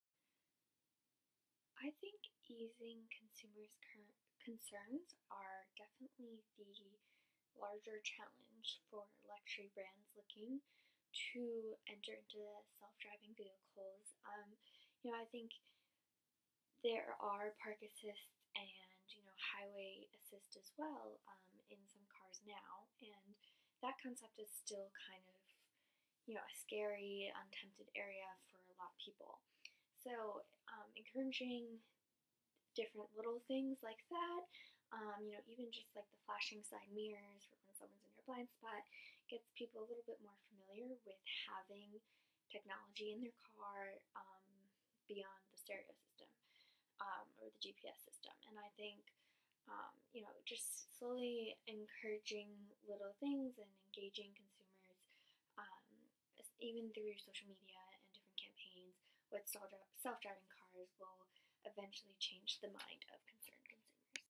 35pc of US drivers believe autonomous vehicles are the future of driving: report
35pc of US drivers believe autonomous vehicles are the future of driving: report